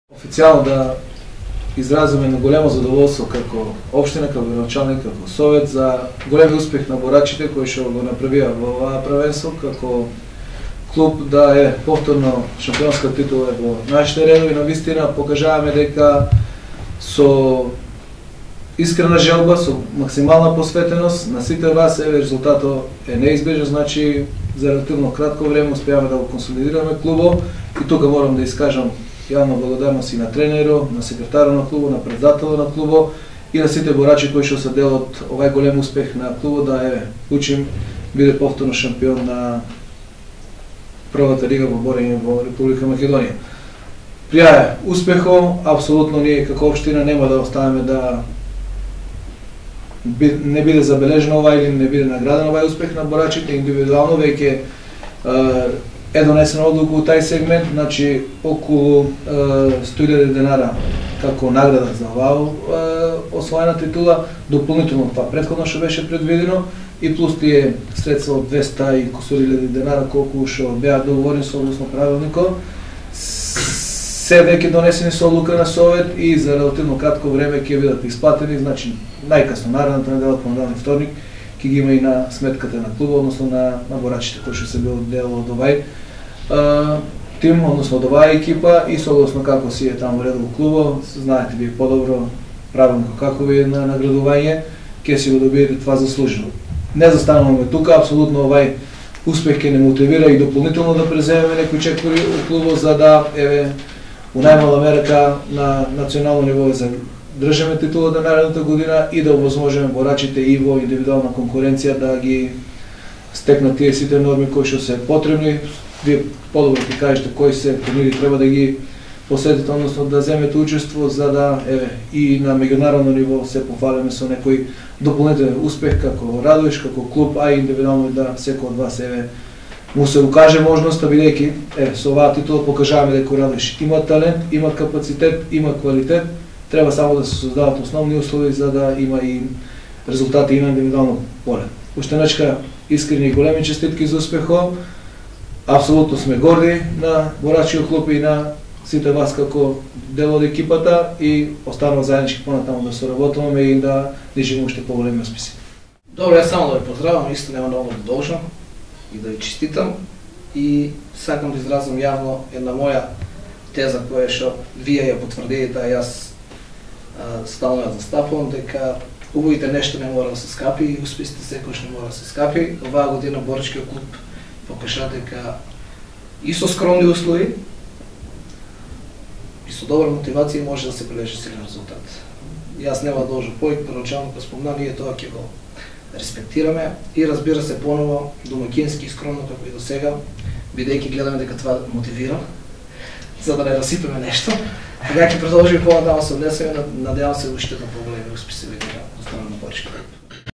БОРАЧИТЕ НА БК БУЧИМ НА ПРИЕМ КАЈ ГРАДОНАЧАЛНИКОТ САШКО НИКОЛОВ
Денеска на прием кај Градоначалникот на општина Радовиш беа борачите од БК Бучим кој минатата недела ја освоија шампионската титула на Македонија за 2013 година.